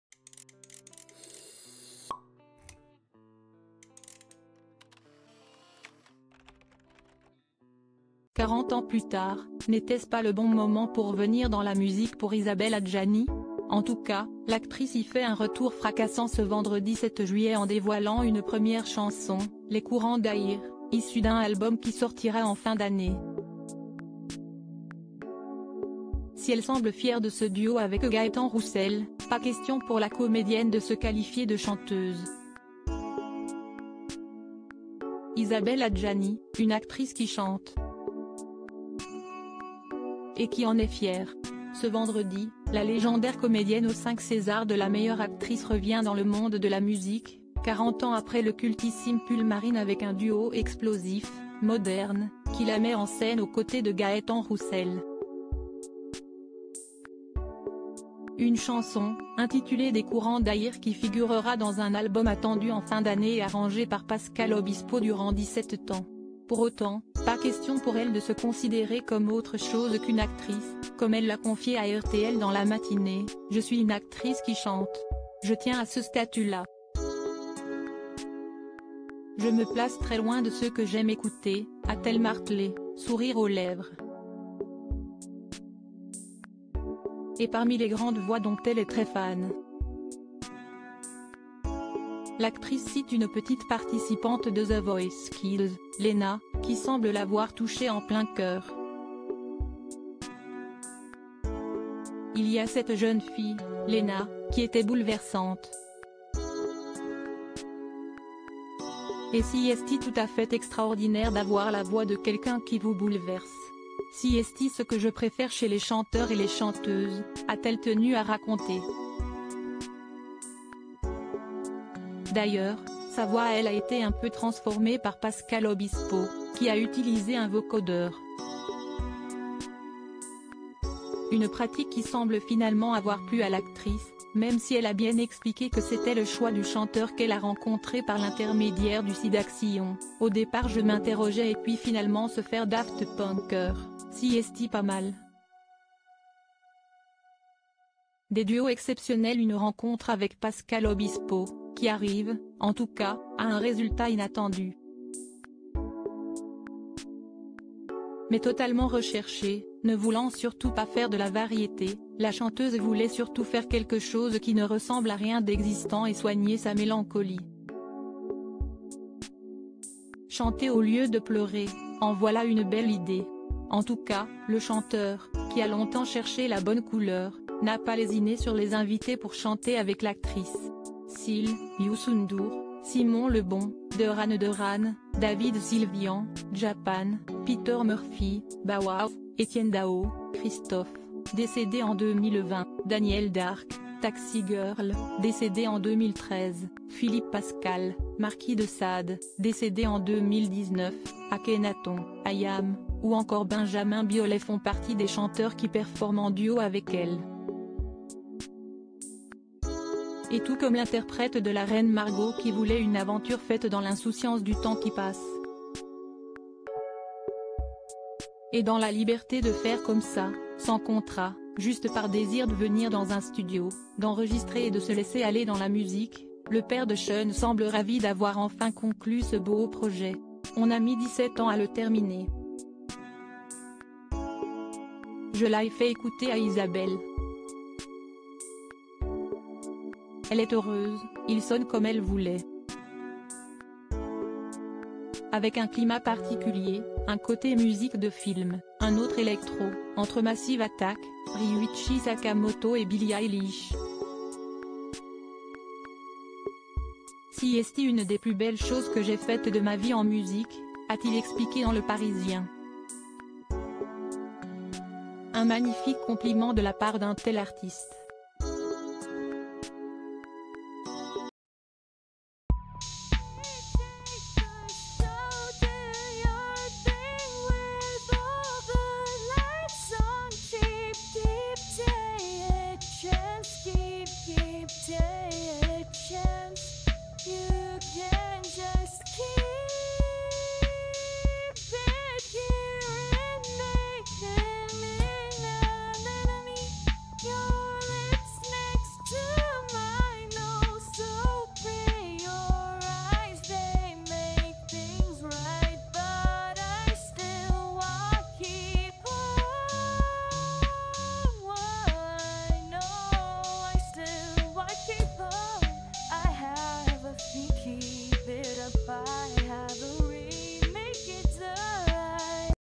"Être Daft Punkée, c'est pas mal !" : Isabelle Adjani une "actrice qui chante", confidences sur son
"Être Daft Punkée, c'est pas mal !" : Isabelle Adjani une "actrice qui chante", confidences sur son retour musical